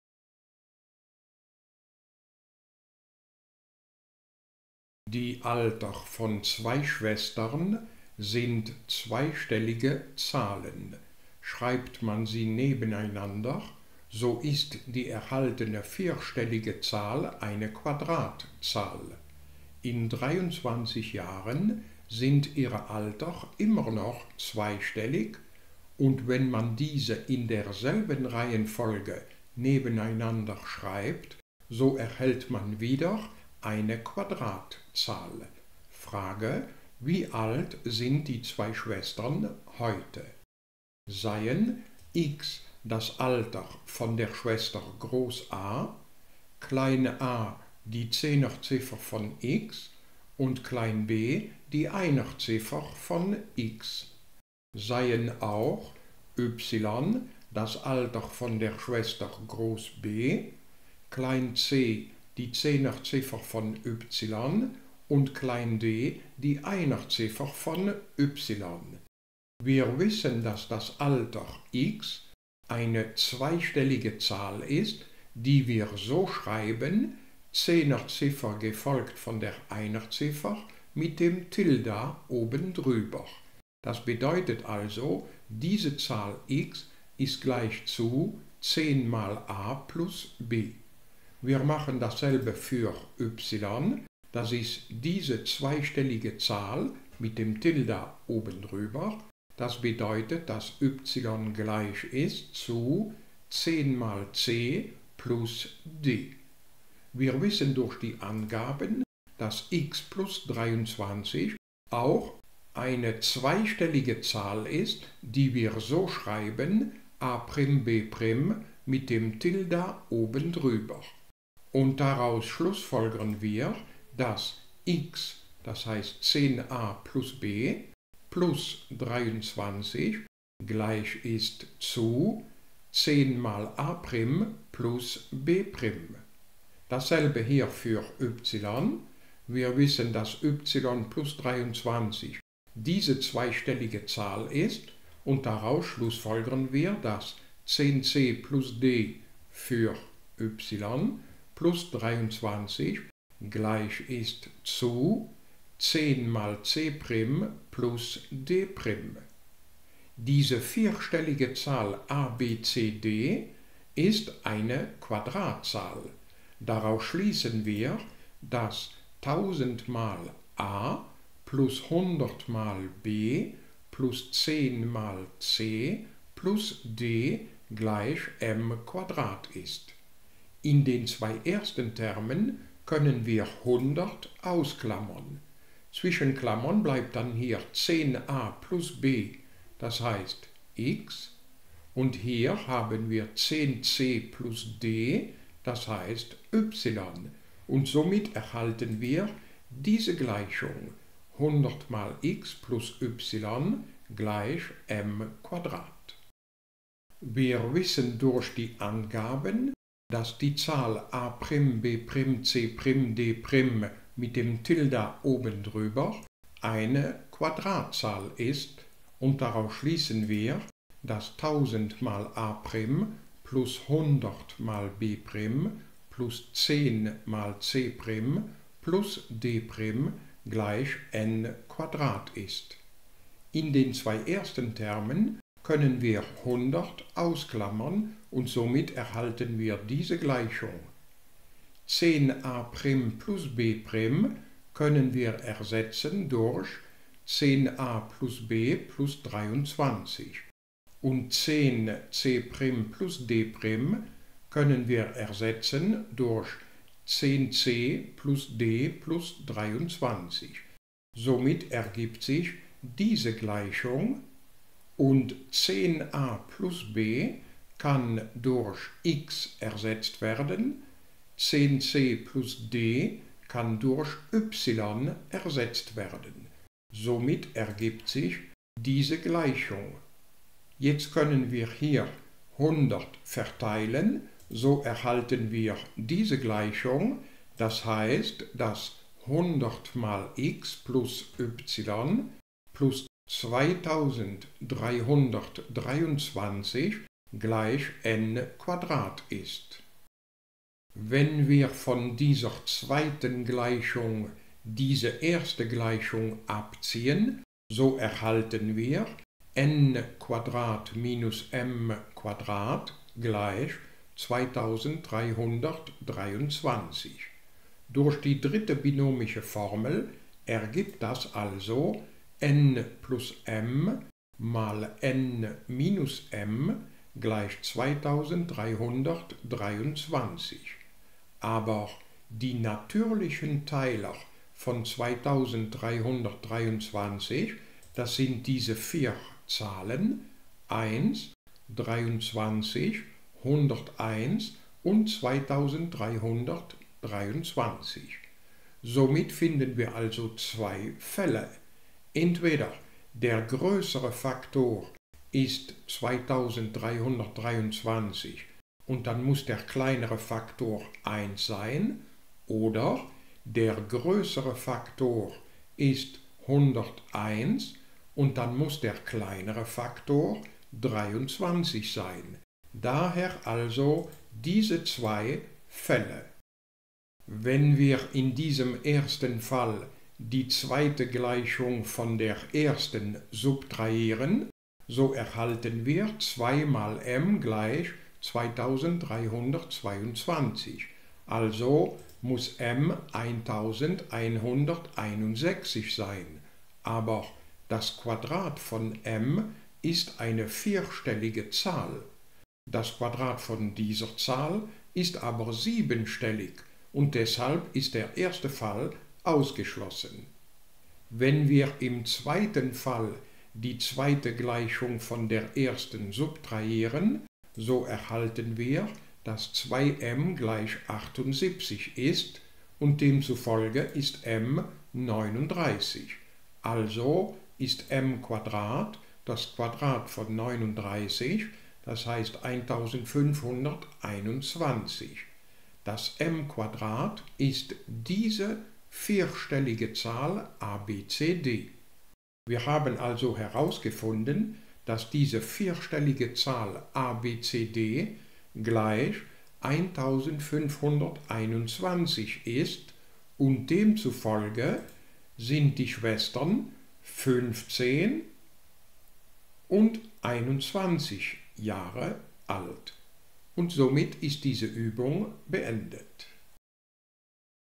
Mathematik-Olympiade Finale 2023 Midi 081
Wir lösen eine Aufgabe aus dem Finale der belgischen Midi-Mathematik-Olympiade 2023.